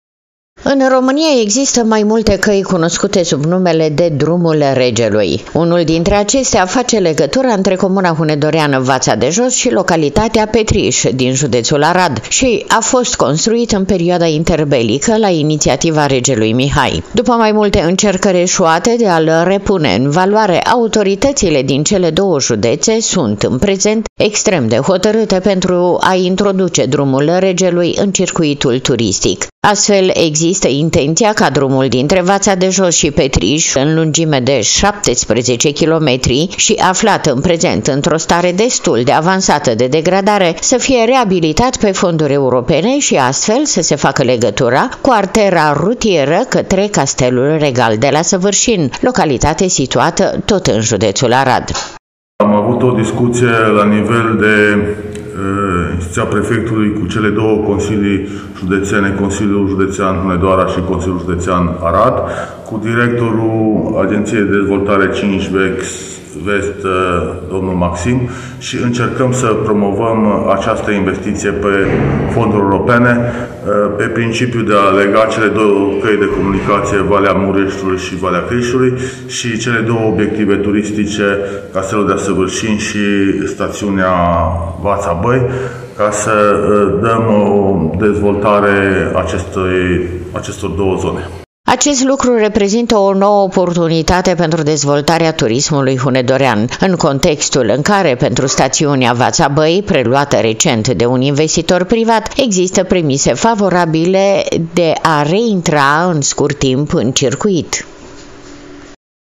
“DRUMUL REGELUI”, REABILITAT CU FONDURI EUROPENE
În România există mai multe căi cunoscute sub numele de “Drumul Regelui”. Unul dintre acestea face legătura între comuna hunedoreană Vaţa de Jos şi localitatea Petriş din judeţul Arad și a fost a construit în perioada interbelică, la inițiativa Regelui Mihai.